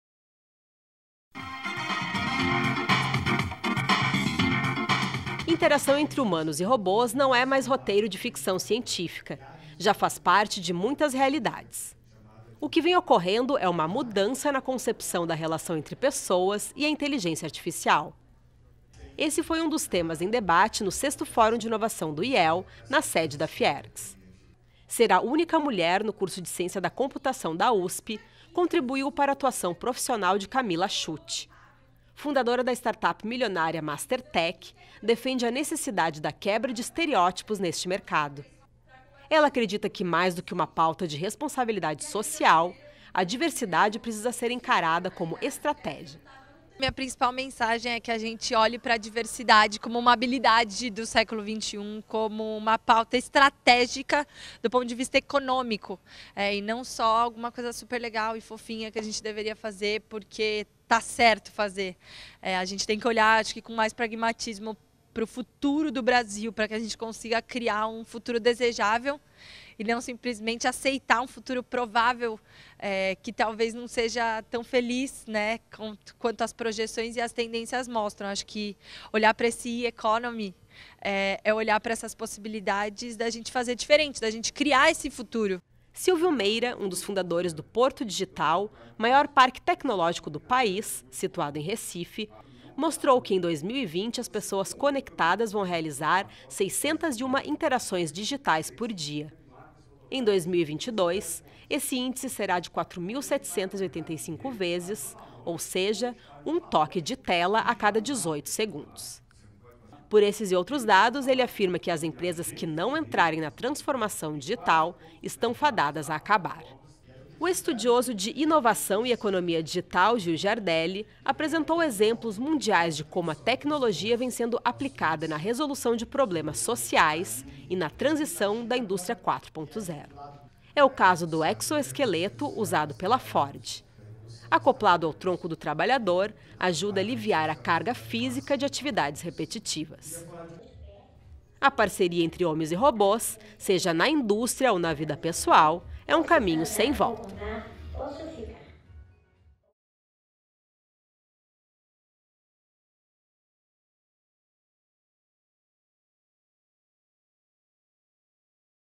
Fórum IEL de Inovação debate a transformação digital
Acabou o mundo V.U.C.A (volatilidade, incerteza, complexidade e ambiguidade) e começou a era da economia da inteligência artificial. Esta foi a primeira frase de Gil Giardelli no 6º Fórum IEL de Inovação, realizado nesta quarta-feira, na sede da FIERGS. “A automação e os robôs são sinônimo de mais empregos e não de desemprego”, afirma ele, que é professor e colaborador de instituições como Insper, Fundação Dom Cabral e PUC-SP. Conforme Giardelli, países com pleno emprego são as nações que pensam o futuro, independente de eleições ou governos.